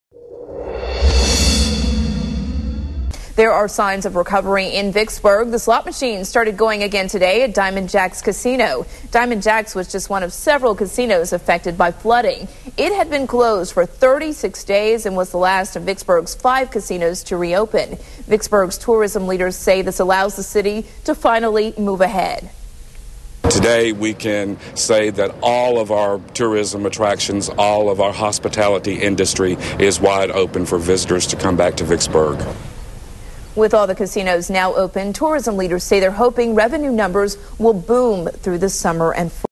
DiamondJacks Reopens In Vicksburg
DiamondJacks reopens in Vicksburg, more than a month after it closed due to flood concerns.